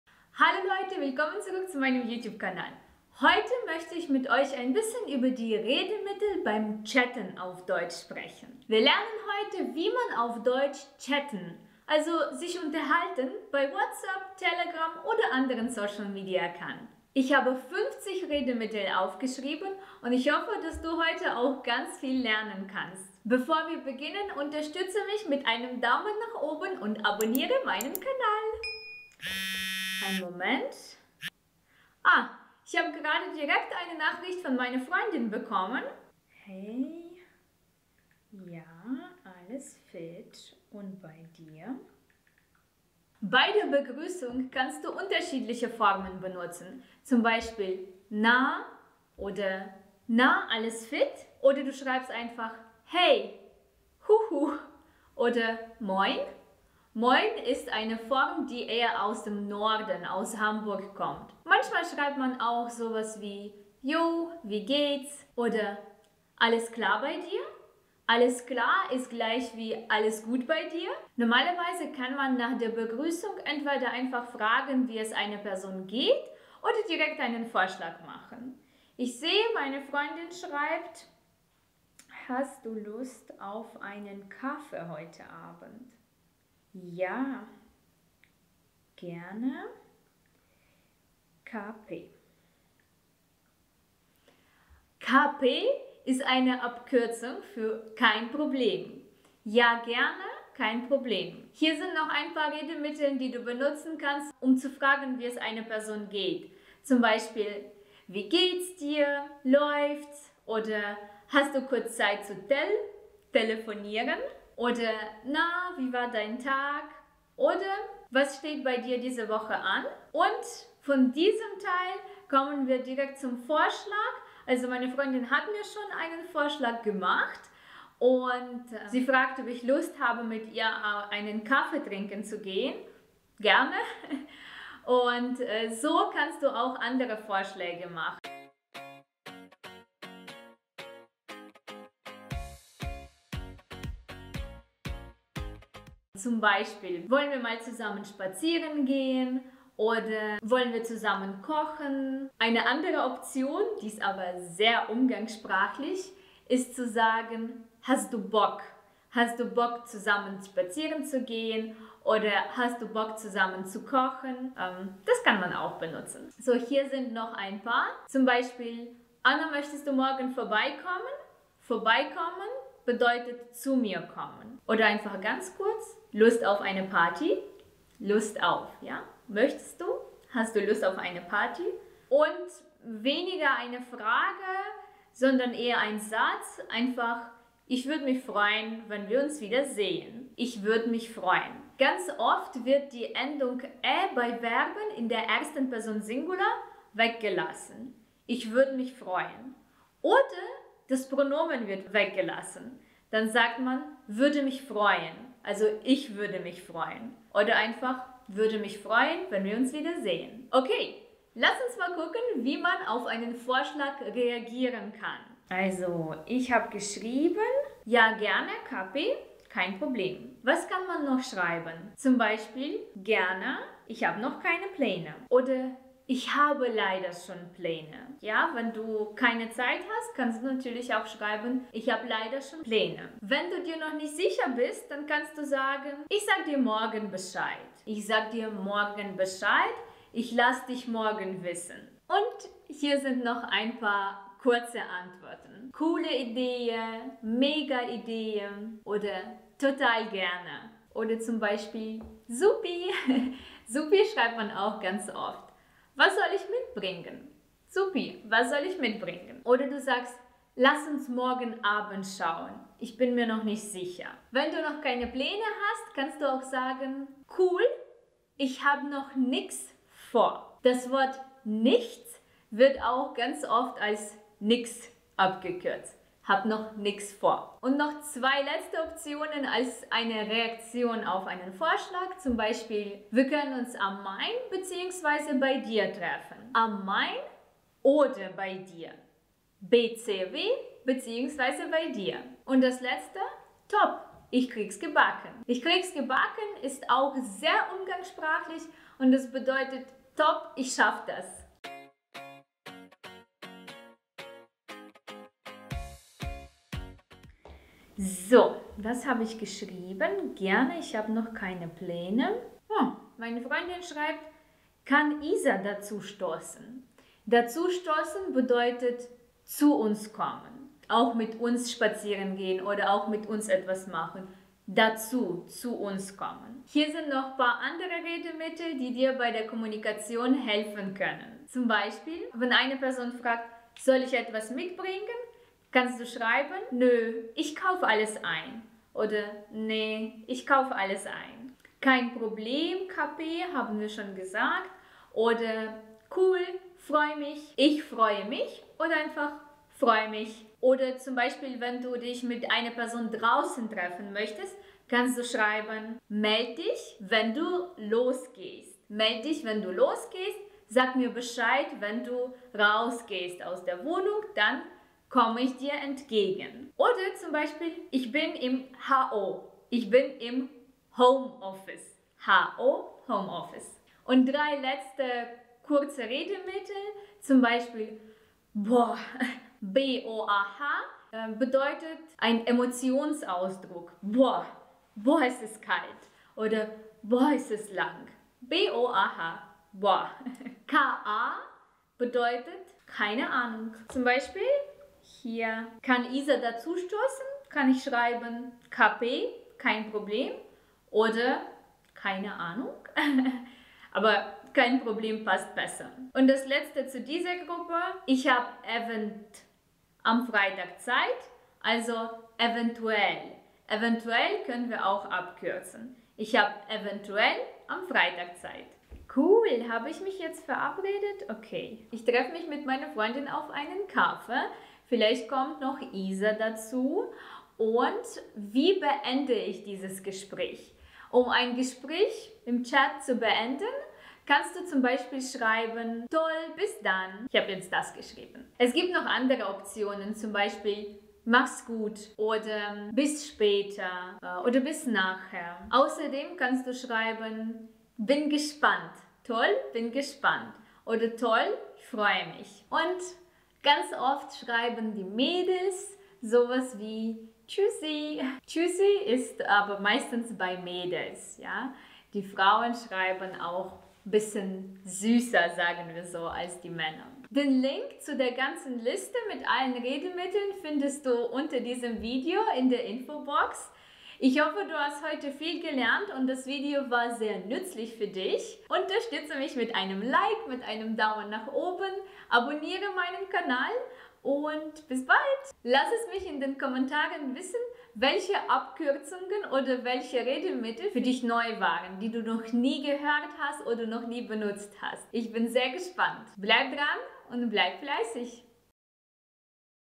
TOP 50 Redemittel fürs CHATTEN | Learn German Fast
📌Hast du dich schon mal dabei ertappt, dass du einige Redemittel nicht verstehst? Gibst du dir Mühe, beim Chatten entspannt und modern zu klingen? Dann ist das Video genau richtig für dich 🎯
❗️Hier gebe ich dir die TOP 50 Redemittel, die du direkt beim Chatten benutzen kannst.❗️
Wie immer, findest du die komplette Liste auf meiner Seite. KOSTENLOS.❤️

👉 Bleibt dran und bleibt fleißig!

📗Grammatik Aktiv A1-B1

📗Grammatik Aktiv B2-C1

📗Lerngrammatik zur Studienvorbereitung